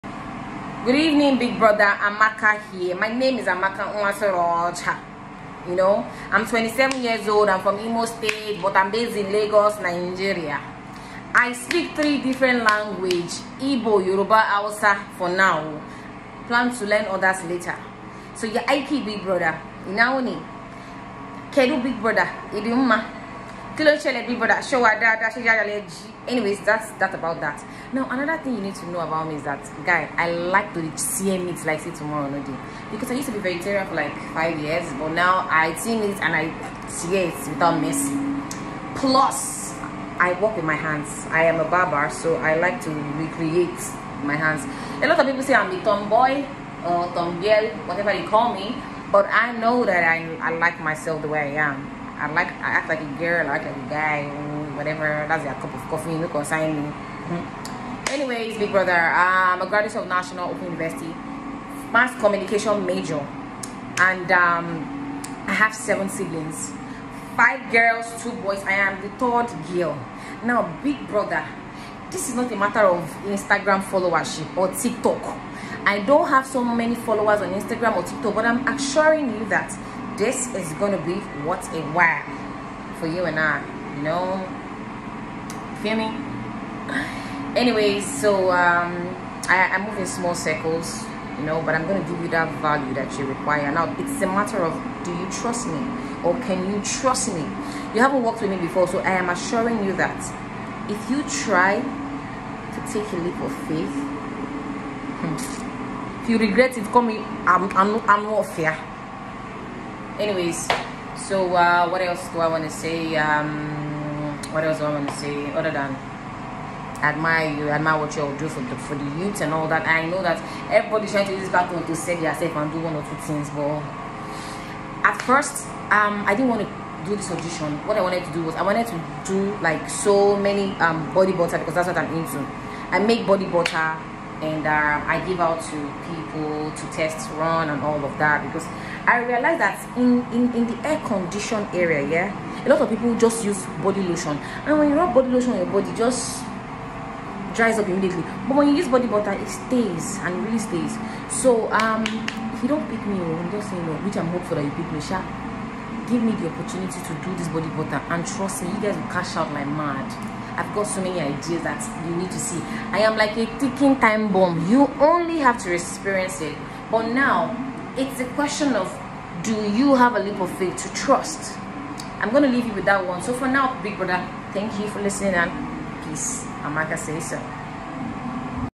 BEST WAY TO INTRODUCE YOURSELF FOR BIG BROTHER NAIJA AUDITION——season 7
Understand that this is a reality show.
Be real
Be concise
Be natural and always remember keep a smile